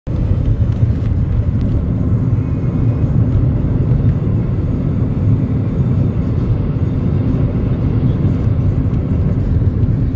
EDSA Kamuning Footbridge
Derisively referred to as the "tallest footbridge in the world" due to its unfriendly design.